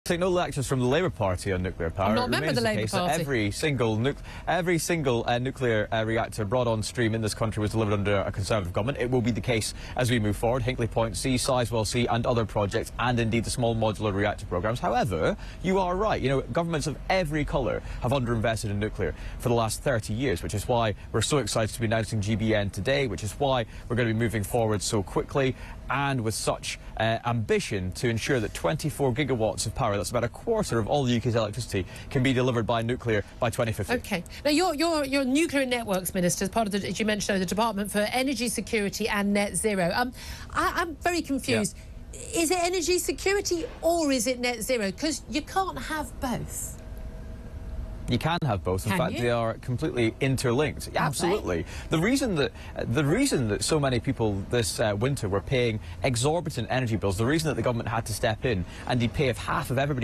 Julia Hartley-Brewer And Conservative Nuclear Minister CLASH Over Net Zero
Julia Hartley-Brewer And Conservative Nuclear Minister CLASH Over Net Zero On Talk TV;